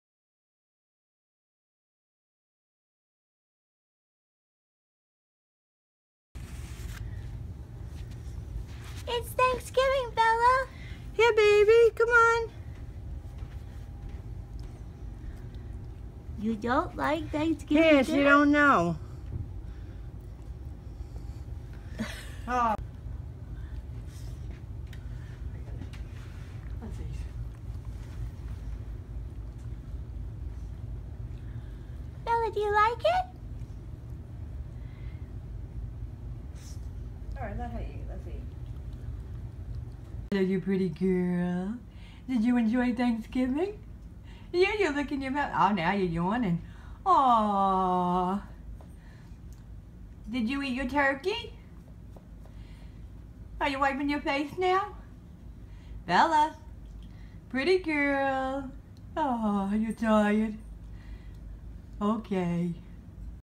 Bella the rescue dog - First Thanksgiving
Bella is a 3 year old poodle mix who was adopted on 1/6/16 after being found as a stray. This was her first Thanksgiving with us. The food on the plate was all safe for her to eat, we made absolutely sure before we prepared and gave it to her. She was extremely thin when she came home with us, but as you can see she filled out well!